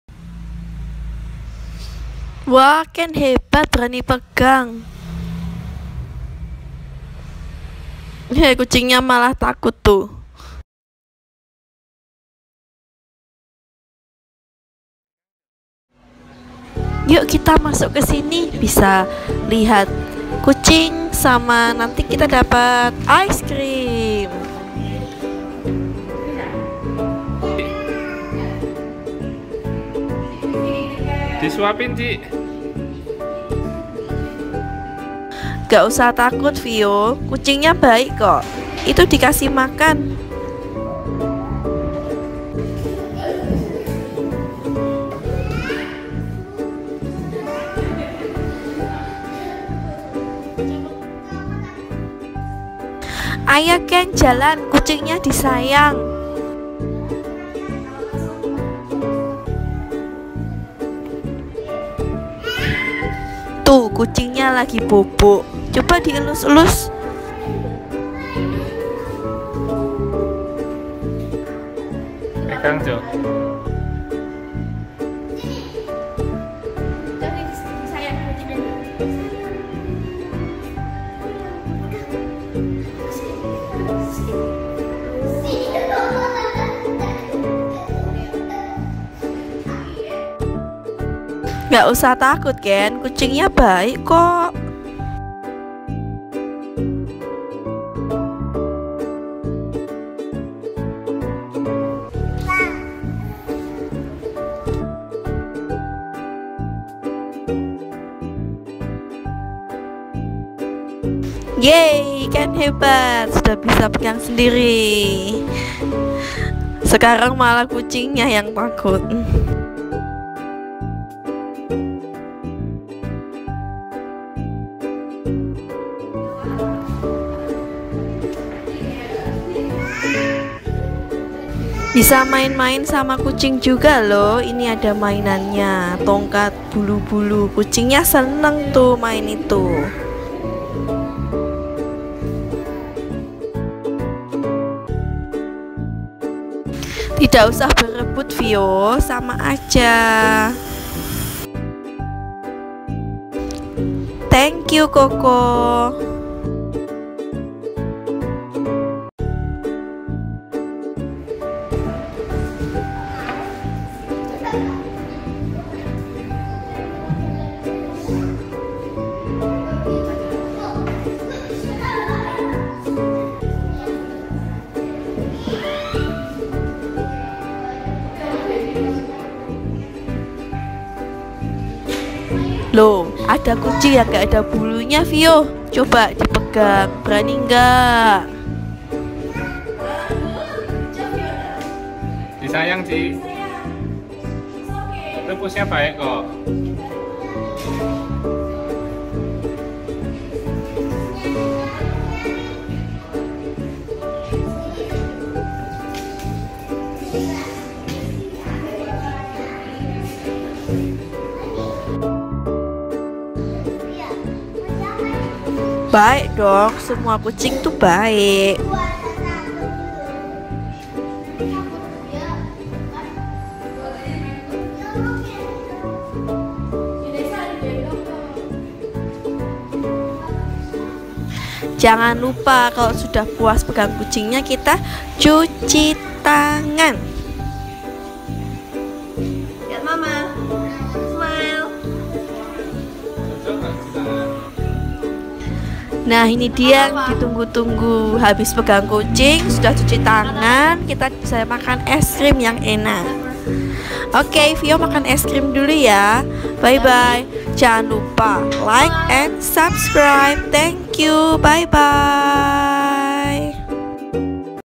CAT CAFE AND ICE CREAM
Miaw.. miaw... 🐱🐱
Waaaww,, Fiona and Kenzo go to cat cafe to spend time with their cute cat and enjoy delicious ice cream...